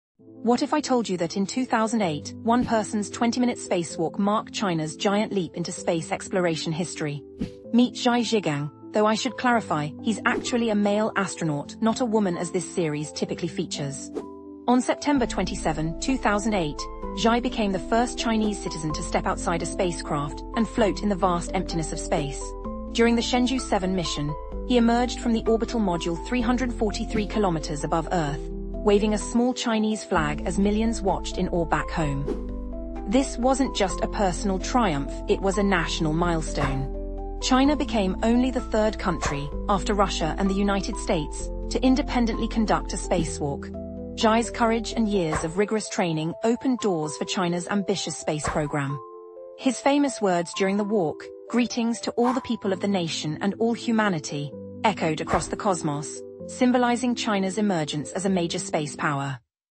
China's First Spacewalk Pioneer
Zhai Zhigang: First Chinese astronaut to perform a spacewalk

What if I told you that in 2008, one person's twenty-minute spacewalk marked China's giant leap into space exploration history? Meet Zhai Zhigang, though I should clarify - he's actually a male astronaut, not a woman as this series typically features.

On September 27, 2008, Zhai became the first Chinese citizen to step outside a spacecraft and float in the vast emptiness of space. During the Shenzhou 7 mission, he emerged from the orbital module 343 kilometers above Earth, waving a small Chinese flag as millions watched in awe back home.

This wasn't just a personal triumph - it was a national milestone. China became only the third country, after Russia and the United States, to independently conduct a spacewalk. Zhai's courage and years of rigorous training opened doors for China's ambitious space program.

His famous words during the walk, 'Greetings to all the people of the nation and all humanity,' echoed across the cosmos, symbolizing China's emergence as a major space power.